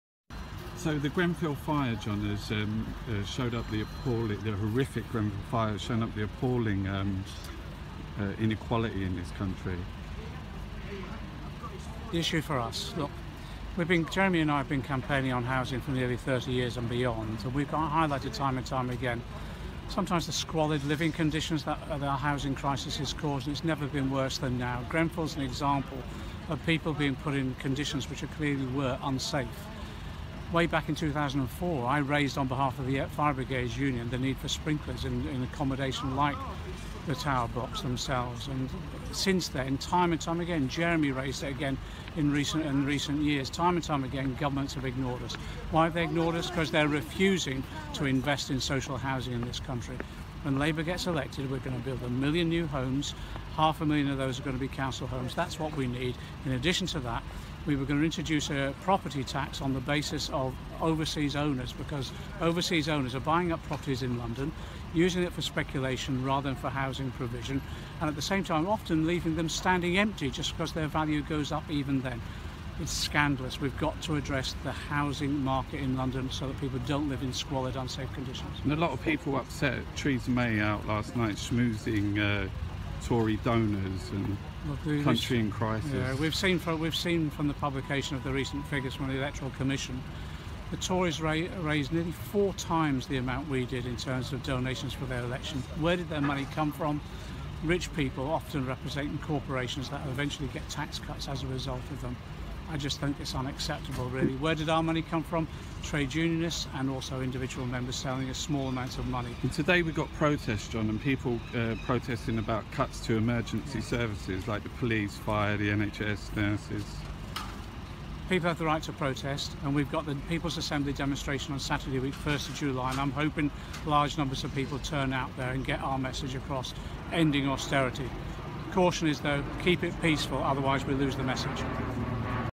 MP John McDonnell Commits To Installing sprinker systems in all Public Owned Housing Blocks
MP John McDonnell has committed the Labour Party to installing sprinkler systems in all public housing blocks as well as fixing them up to surpass modern fire standards.